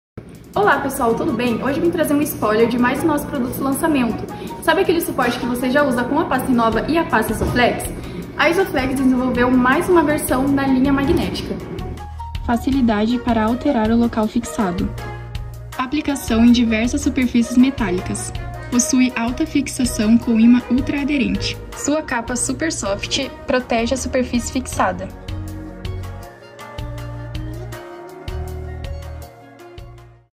Suporte Magnético - Isoflex
Mais um produto para otimizar os seus resultados. Aquele que você já conhece, agora em nova versão: Suporte de Pastas Magnético! 🧲

Confira mais detalhes no vídeo! 📹